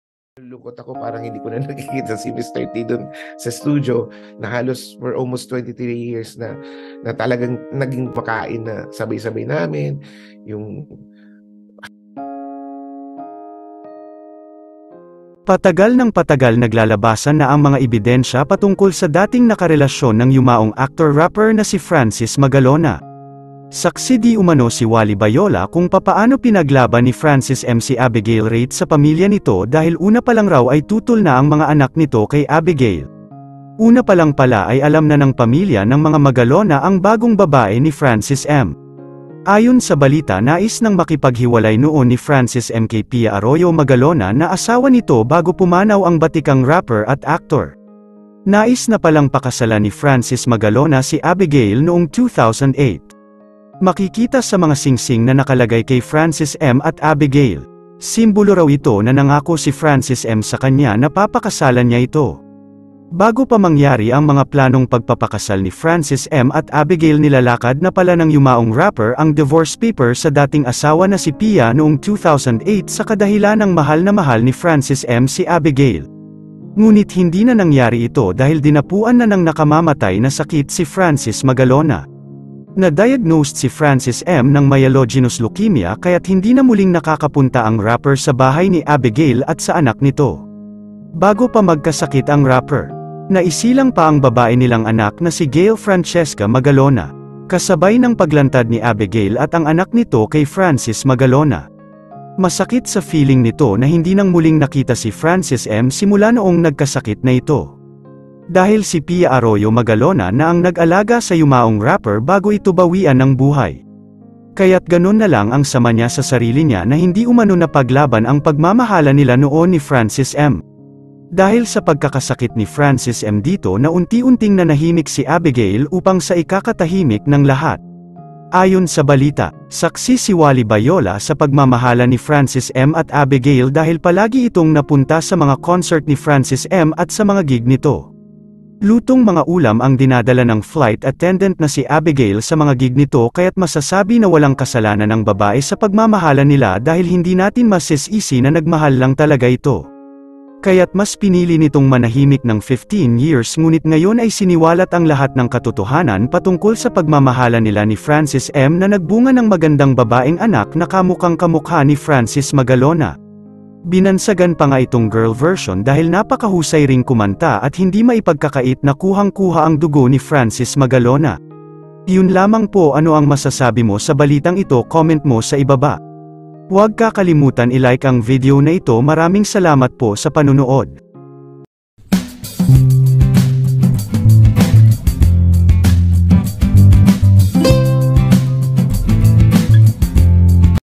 Wally Bayola SAKSI sa PAGMAMAHALAN ni Francis Magalona at Abegail Rait BFF PALA SILA!
Saksi pala si Wally Bayola sa pagmamahalan ni Francis Magalona at Abegail Rait at ang mahal na mahal na anak sa labas na si Gail Francesca Magalona

Warning: This youtube channel provides information on the news of their favorite celebrities providing educational purposes and awareness to our viewers.